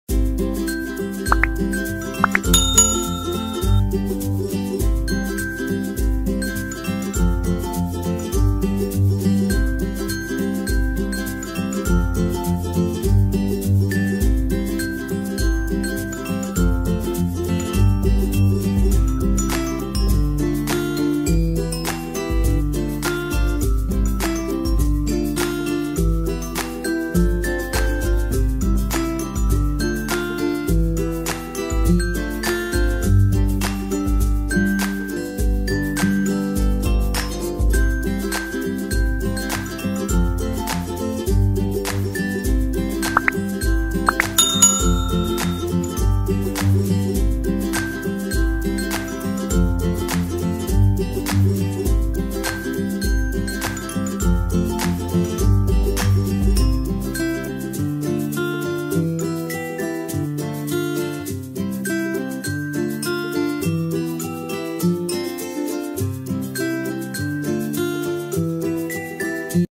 Carrot and dates cake
It’s an easy and tasty recipe, so try it and you’ll love it
It’s a carrot cake with an addition of dates to enhance the taste